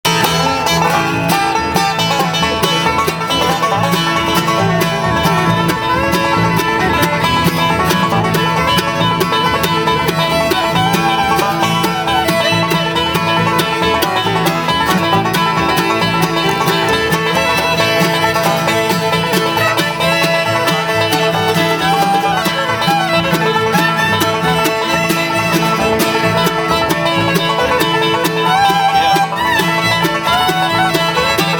Floyd Virginia Highschool bluegrass
Highschool teenagers carrying on the Floyd Virginia musical tradition.